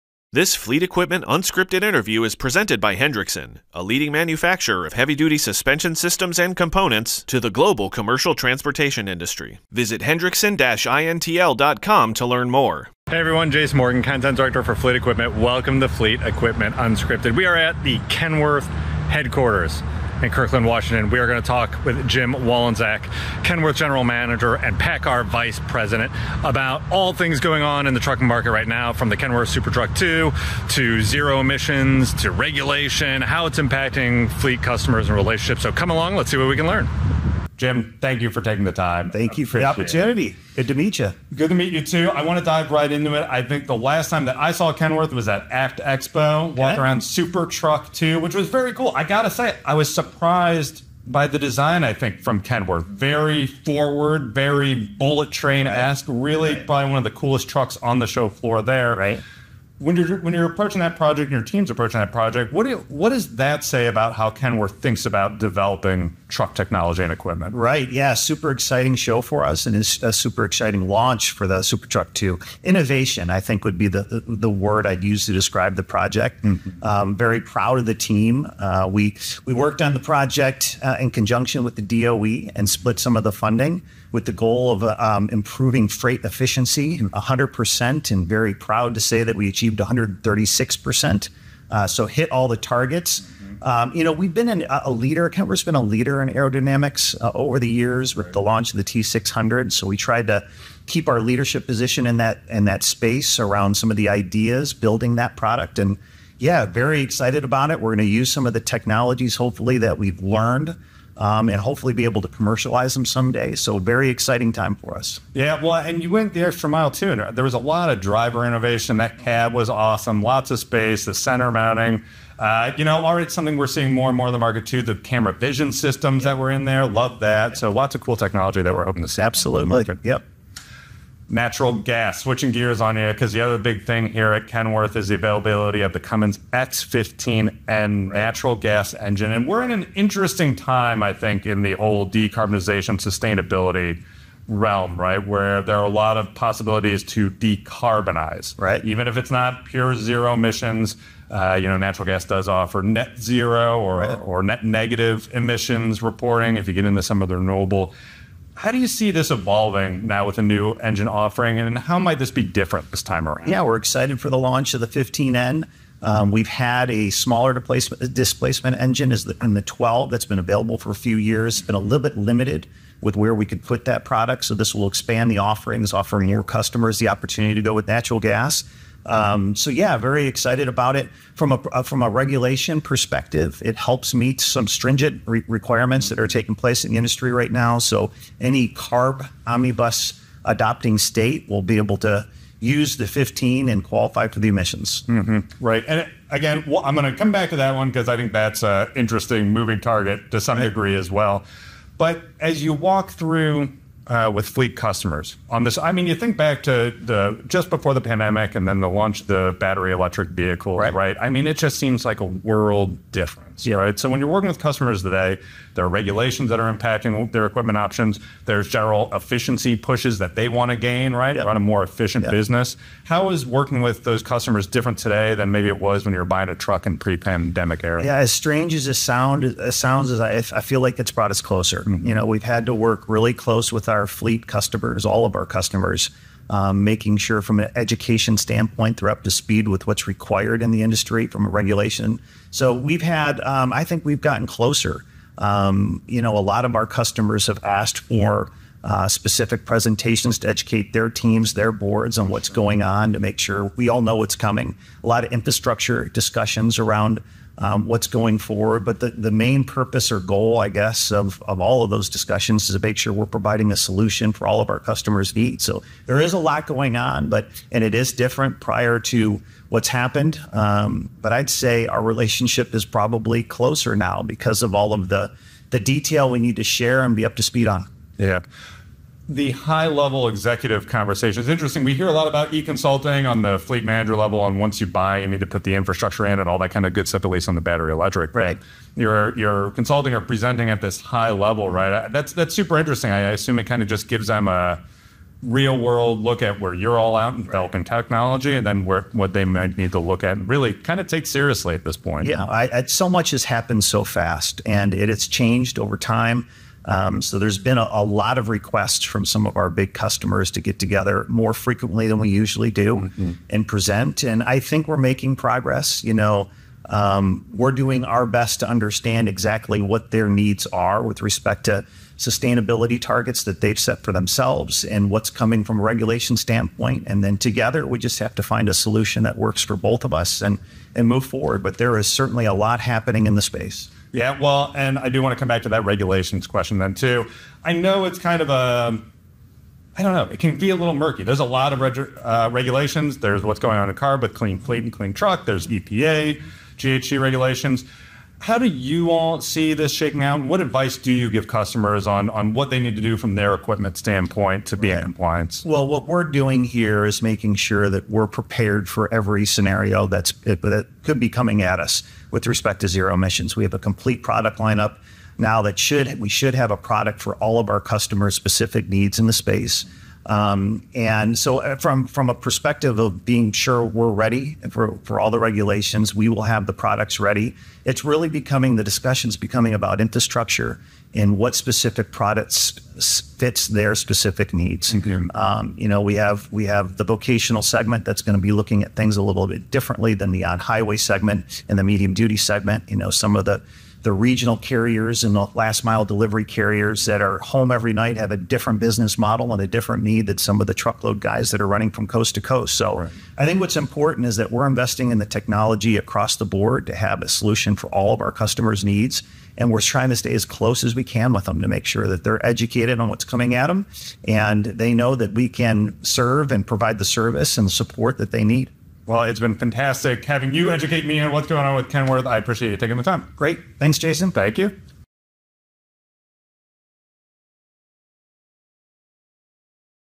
Kenworth General Manager, PACCAR VP discusses truck development
How are we going to move toward increasingly sustainable trucking while keeping fleet operations profitable? That's the implicit question that Kenworth's latest run of truck equipment announcements and trade show displays have sought to answer. From the bullet train-inspired Kenworth SuperTruck 2 with its PACCAR MX-11 engine, rated at 440 HP, driven through a PACCAR TX-12 automated transmission and sporting a "mild hybrid" 48-volt electric generator using lithium-ion batteries, to the Cummins X15N natural gas-powered T680, to the promise of a hydrogen fuel cell Class 8 truck coming soon, there are no shortages of sustainability and decarbonization-minded truck offerings.

"What we're doing here is making sure that we're prepared for every scenario that could be coming at us with respect to zero emissions," explained Jim Walenczak, Kenworth general manager and PACCAR vice president. "We have a complete product lineup that has a solution for all of our customers' specific needs in the space. From a perspective of being sure we're ready for all the regulations, we will have the products ready."